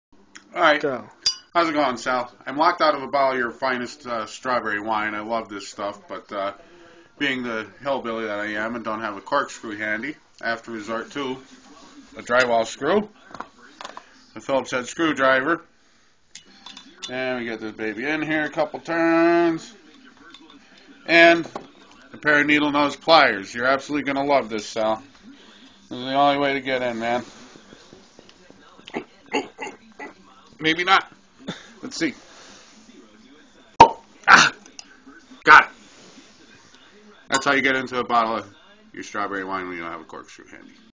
Wine
Sometimes you just have to get into a bottle of wine, but without a corkscrew you can do this!